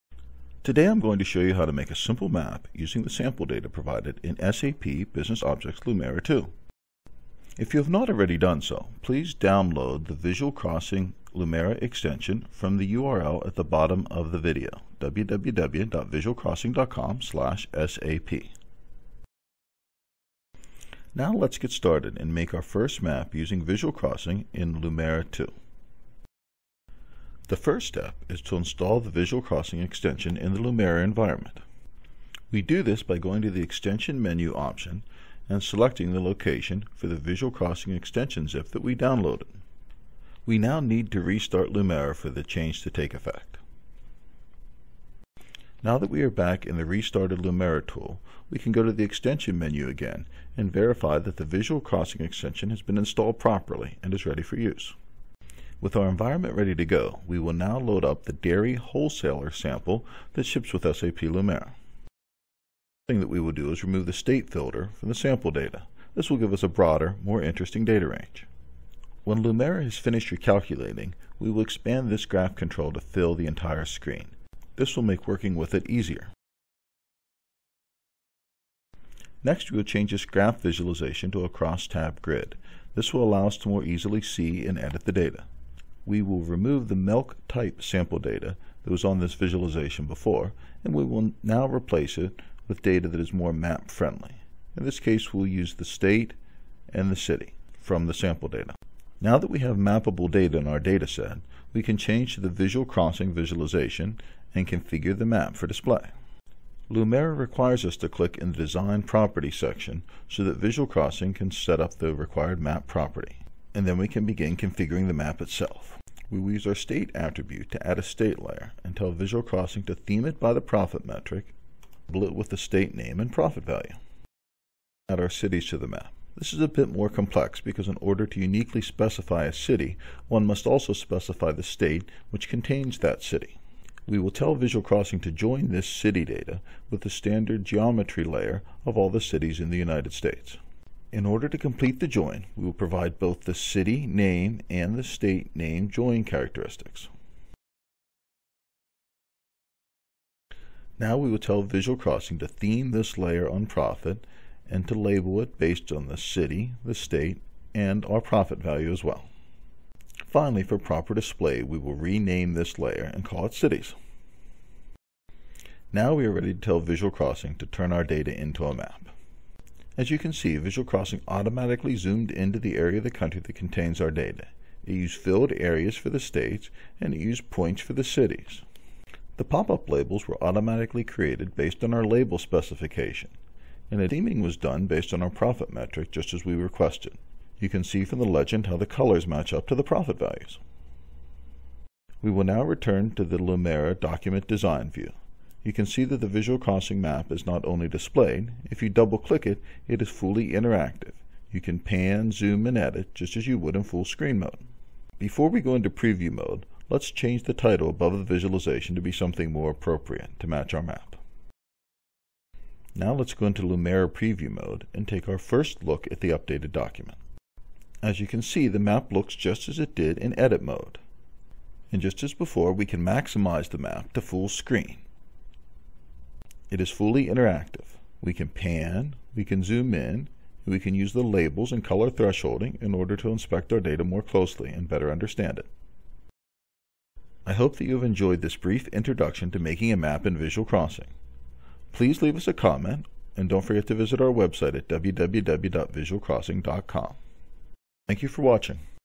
Maps on Lumira Discovery - Getting Started
By following this video any user of SAP Business Objects Lumira Discovery can create their first Visual Crossing map in a few minutes.  This video walks through the process of adding a map into the Dairy Wholesaler example dashboard included as a Lumira Discovery sample.  It starts by installing the Visual Crossing Lumira extension, configuring the business data in Lumira, then configuring the map.  It concludes by showing simple map interactivity in Lumira’s Preview mode.  Anyone who has used SAP Lumira, will find it easy to make a Visual Crossing map in less than five minutes by following this video.